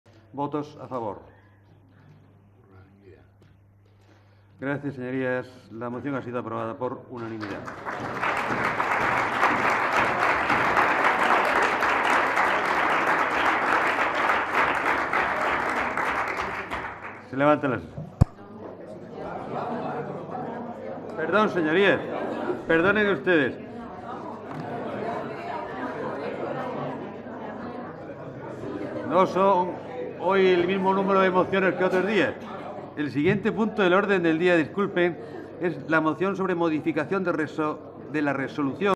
Votación
Votación del dictamen de la Comisión de Competencia Legislativa sobre la solicitud de interposición de recurso de inconstitucionalidad contra la Ley 27/2013, de Racionalización y Sostenibilidad de la Administración Local, formulada por los Grupos Parlamentarios Socialista y Mixto. Y debate y votación de cinco mociones sobre "Apoyo a la declaración de la Fiesta de la Vendimia de Jumilla como Fiesta de Interés Turístico Nacional", de Severa González López, diputada del Grupo Parlamentario Popular; "Solicitud al Gobierno de la Nación para abordar en la Unión Europea el tema de la inmigración como un problema de Estado", formulada por Belén Fernández-Delgado y Cerdá, diputada del Grupo Parlamentario Popular; "Reconocimiento de la Feria de Artesanía de la Región de Murcia FERAMUR como acontecimiento de interés artesano regional", de Ana Aquilino Artero, diputada del Grupo Parlamentario Popular; "Cumplimiento de la resolución número 29 aprobada en el Debate del Estado de la Rgión de 2010 relativa al mantenimiento del hospital del Rosell como segundo hospital del área deCartagena",formulada por Teresa Rosique Rodríguez, del Grupo Parlamentario Socialista, y "Modificación de la resolución por la que se dictan instrucciones para la realización de la evaluación psicopedagógica a fin de informar a los padres de los resultados de las mismas", de María del Carmen Moreno Pérez, diputada del Grupo Parlamentario Socialista.